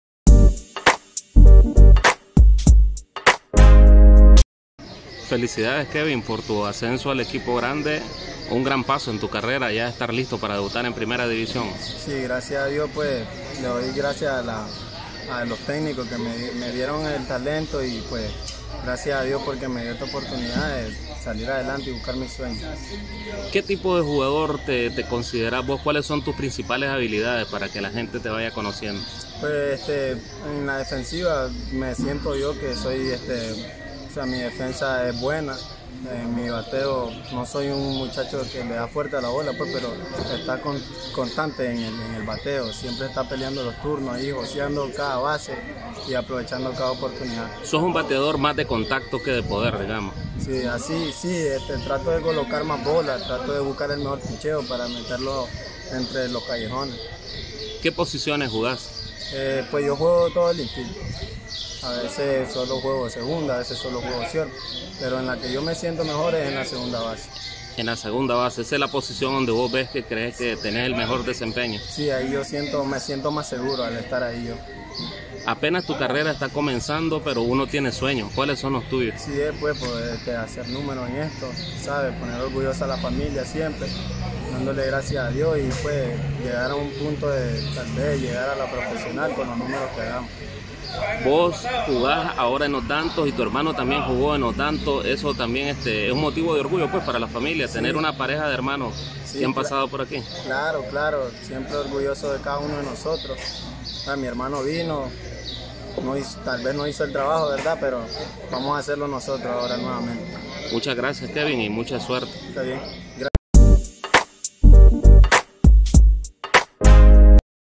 Kevin Aguirre, de 17 años, listo para hacer su debut con la Maquinaria Roja
Es el nuevo prospecto que han ascendido los Dantos al equipo grande e inicia una nueva oportunidad para un chavalo lleno de ilusiones y ganas de sobresalir en el béisbol nacional y más allá.